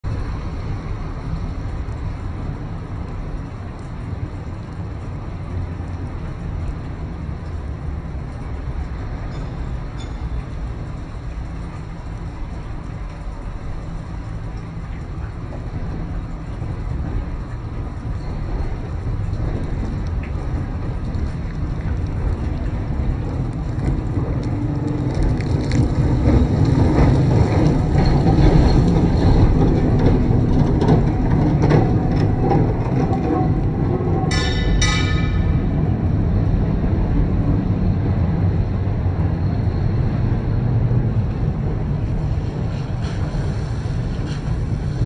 Powell-Hyde Cable Car 10 @ Hyde St & Washington St San Francisco California (Slow Motion)
Cable car in San Francisco, California.

4K 60fps 30fps

March 2023

future publish
cable car